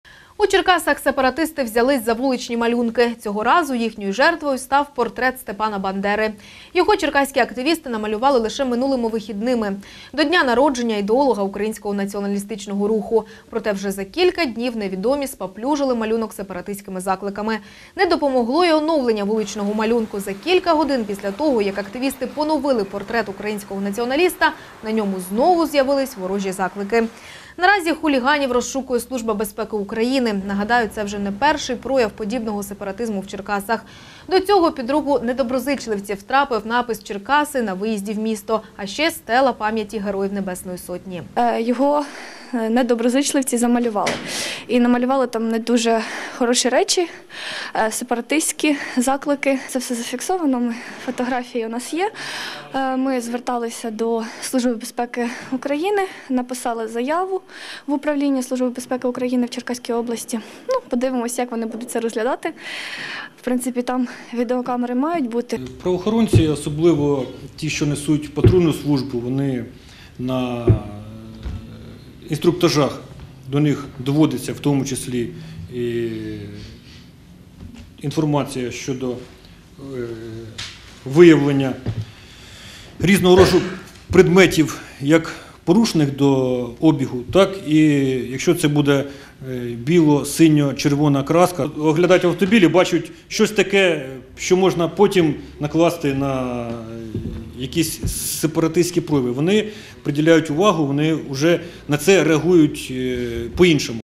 У Черкасах замість націоналістичних графіті з’являються сепаратистські
Подвійна атака. У Черкасах сепаратисти взялись за вуличні малюнки.